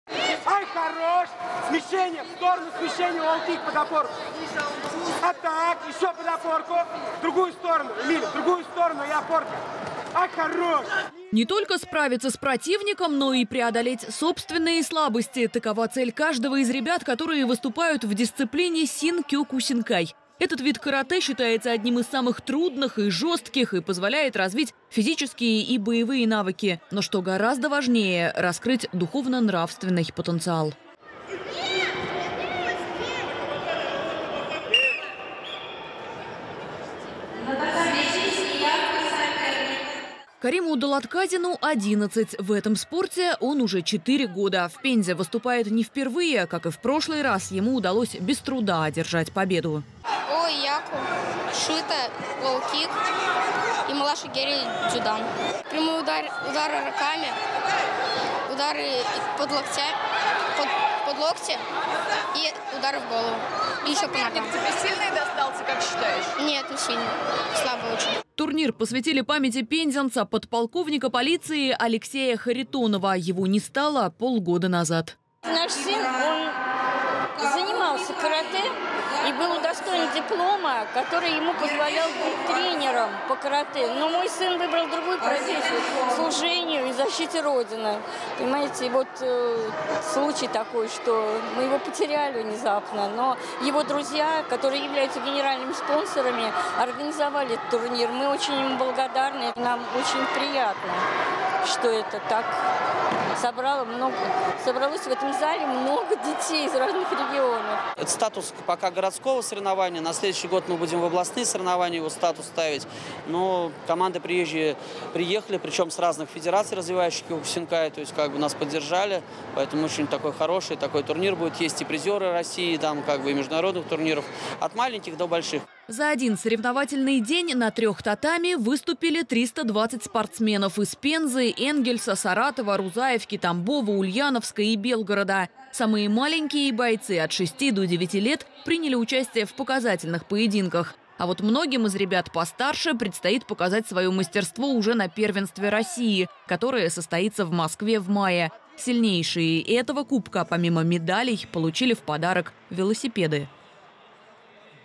В минувшие выходные в Пензу приехали каратисты из шести регионов страны
В областном центре впервые организовали турнир, посвященный памяти подполковника полиции Алексея Харитонова. На церемонии открытия соревнований присутствовали его родные и близкие.